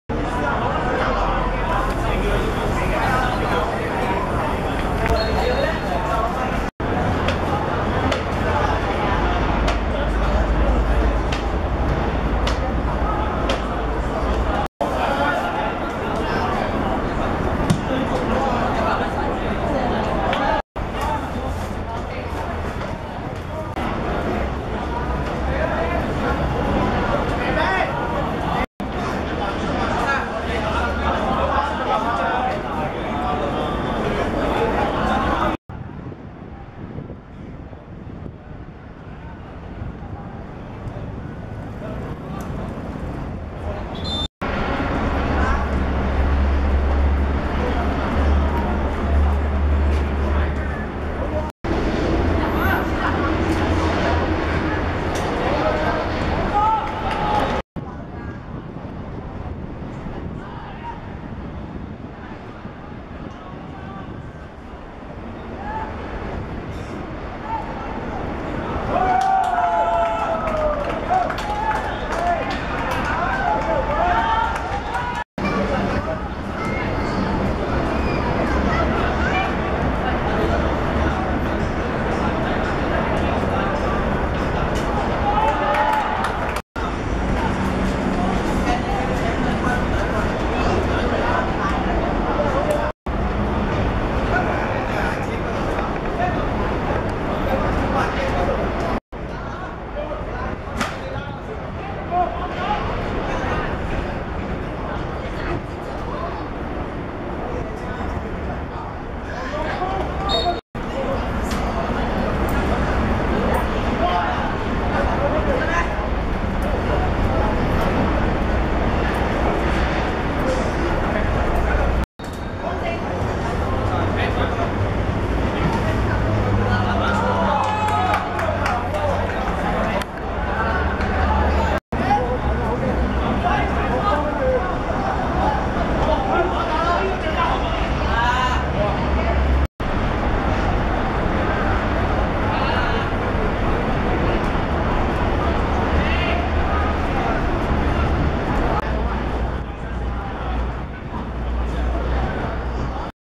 陳黃淑芳vs董玉娣(2015.1.17.屯門學界足球乙組)精華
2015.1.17.兆麟運動塲 10:30 陳黃淑芳(綠) 2:0 董玉娣(白)